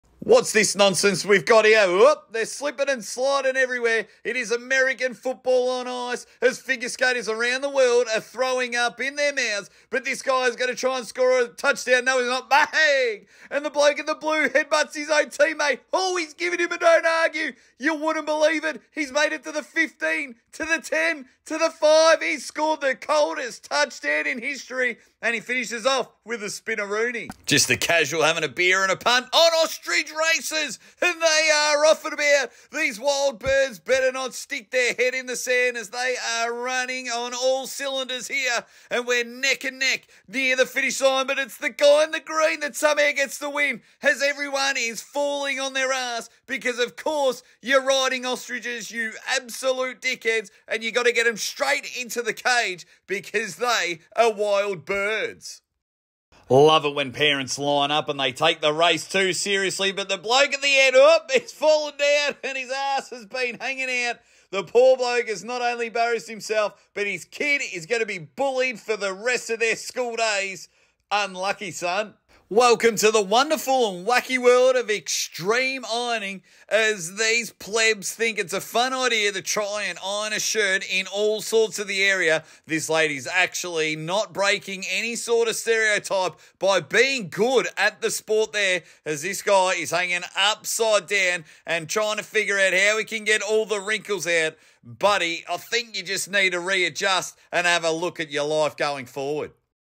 WTF | Sports that will make you laugh
We love sports and we also love wacky sports and in this video we cover some sports that make you go WTF